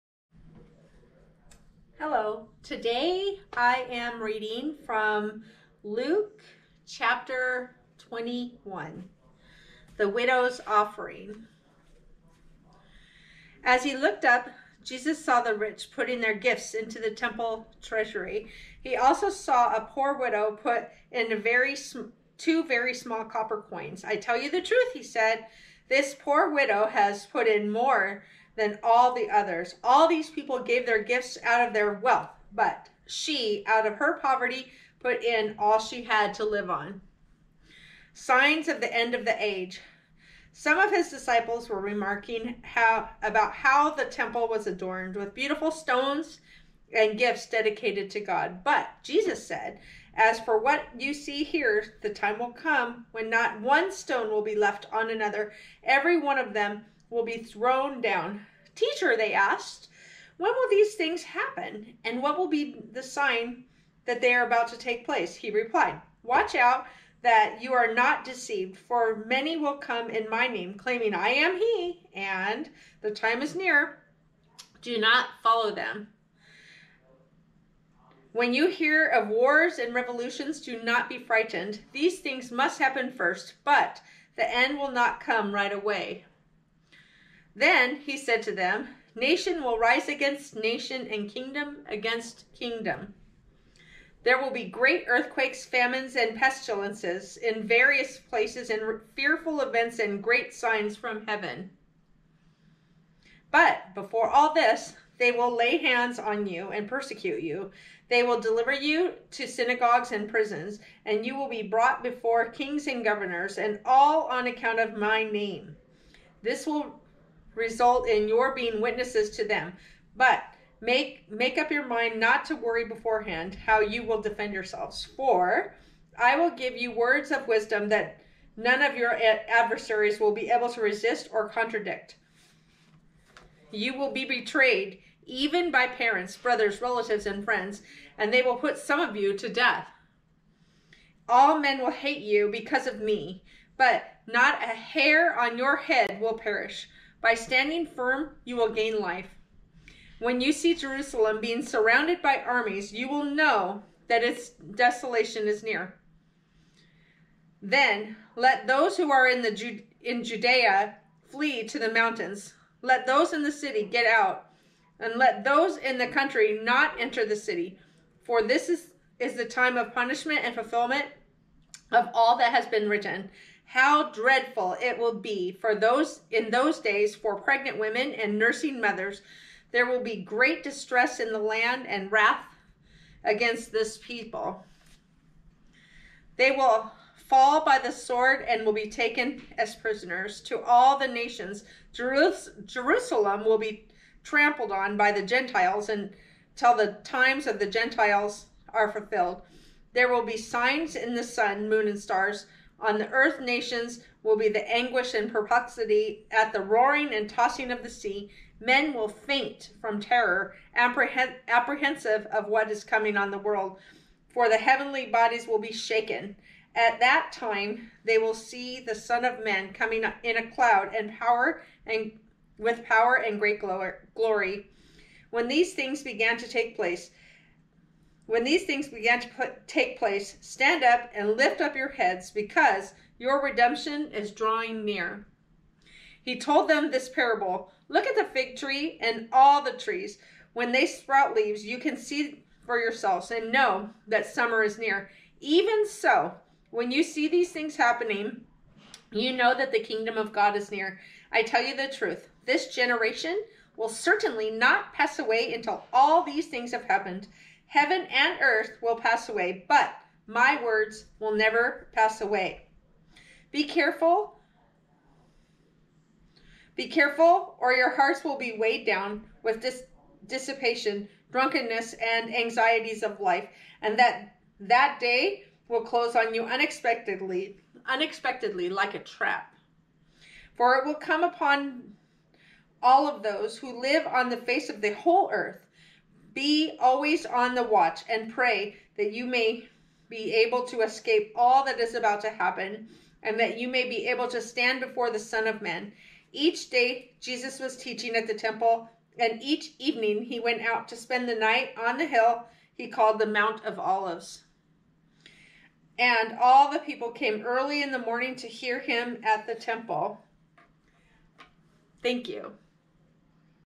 Luke 21, Bible Read Aloud
I have a goal.  My goal is to read the entire New Testament from beginning to end. I want to know Him better by spending consistent time in His word.  I know there are many different plans to complete the entire Bible, but I am going to keep it simple and just start from the New Testament.